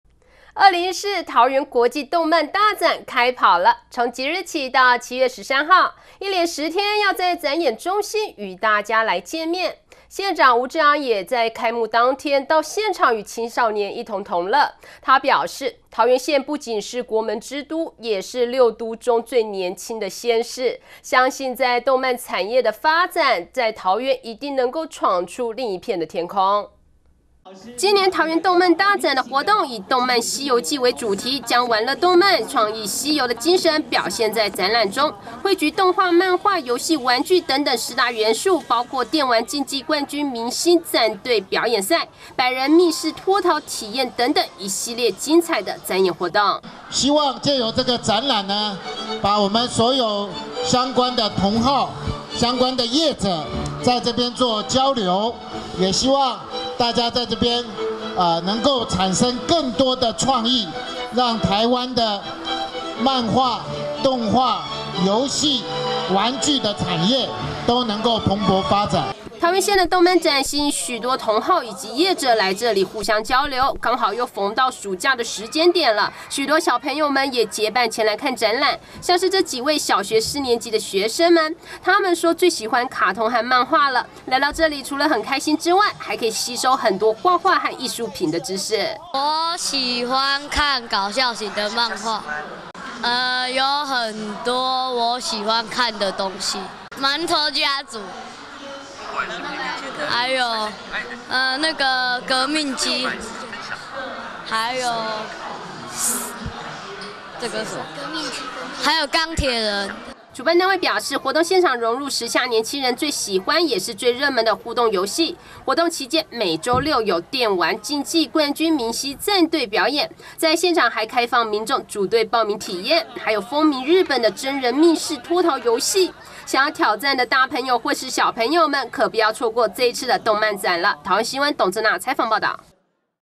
桃園有線新聞1030704 桃園動漫展開跑 四大主題等你挑戰
2014桃園國際動漫大展開跑了，從即日起到7月13號，一連十天要在展演中心與大家見面，縣長吳志揚也在開幕當天，到現場與青少年同樂，他表示，桃園縣不僅是國門之都，也是六都中最年輕的縣市，相信動漫產業的發展，在桃園一定能夠闖出另一片天空。